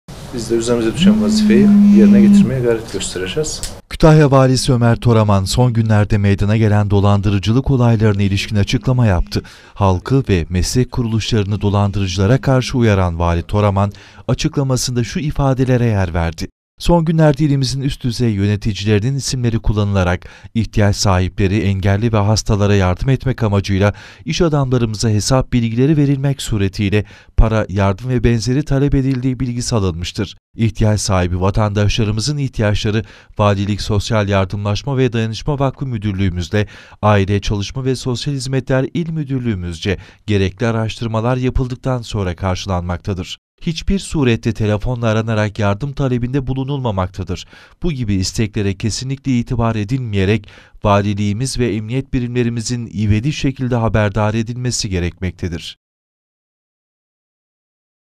VALİ TORAMAN DOLANDIRICILARA KARŞI UYARDI
Haber Metni :

ÜST DÜZEY YÖNETİCİLERİN İSİMLERİNİ KULLANARAK İHTİYAÇ SAHİPLERİNE YARDIM BAHANESİYLE SİZİ ARAYIP HESAP NUMARASI VEREN OLURSA DİKKAT! DOLANDIRILMAK ÜZERESİNİZ. KÜTAHYA VALİSİ ÖMER TORAMAN “HİÇBİR SURETTE TELEFONLA ARANARAK YARDIM TALEBİNDE BULUNULMAMAKTADIR” DEDİ, BU GİBİ İSTEKLERE İTİBAR EDİLMEMESİNİ İSTEDİ.